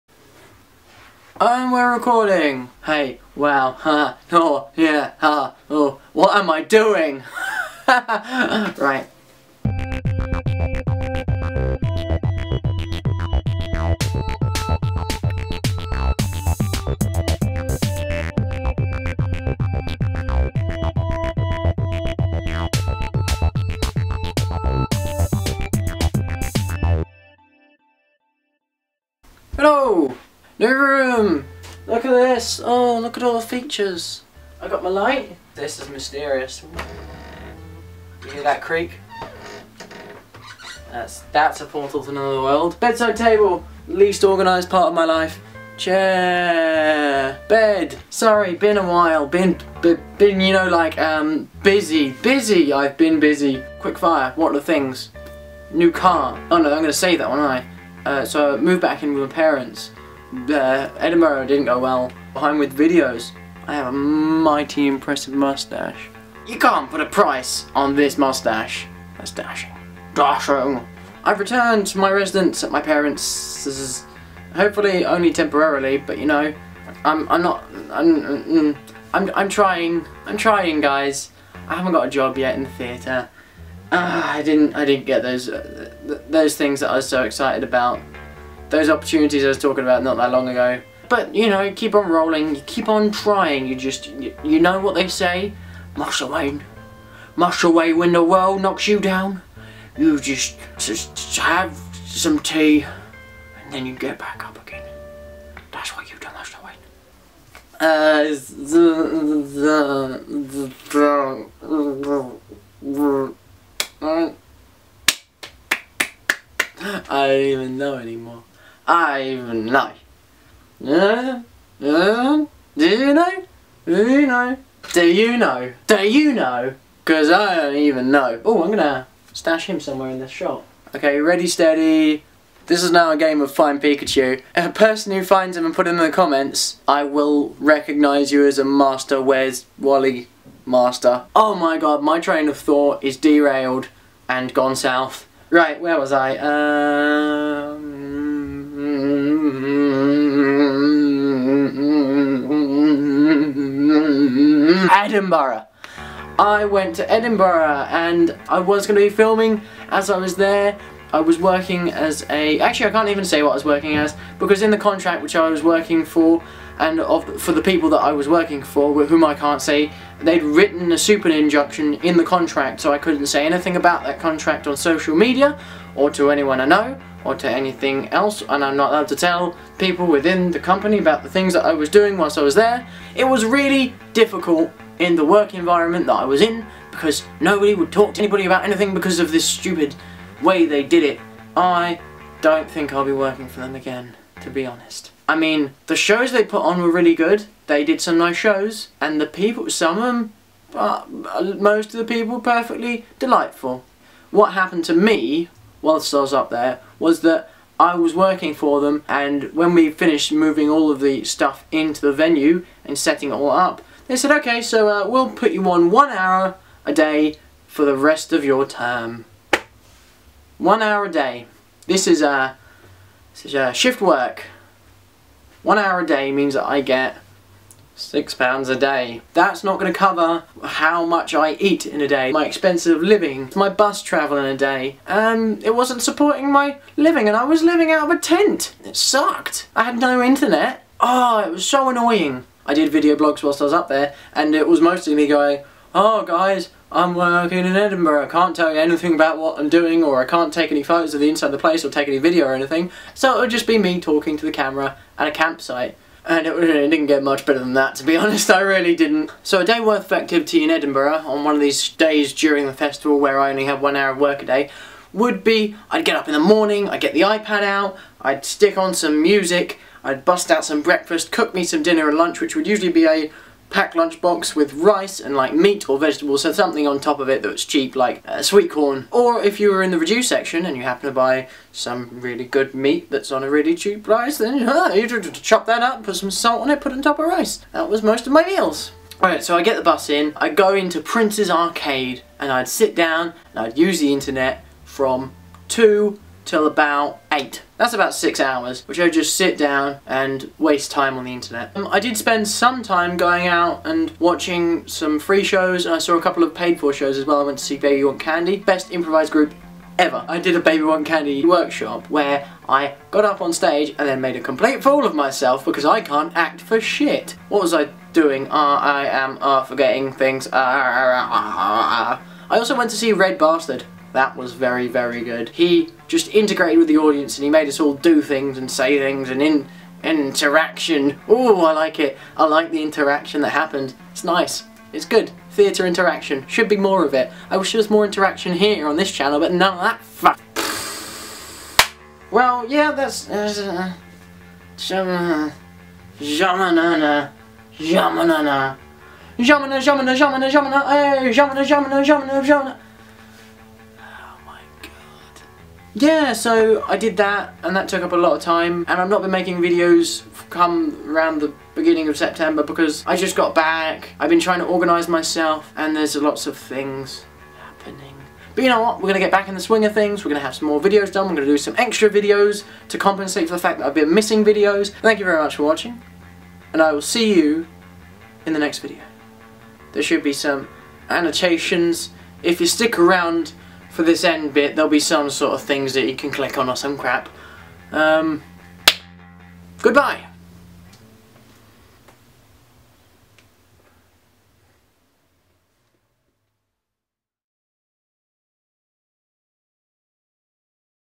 Quickfire Update
this video is what's on in my life as of yet and tanjents and hilarity ensues i talk about my revised living arrangements my escapade in Edinburg, but not about my car as that is coming soon.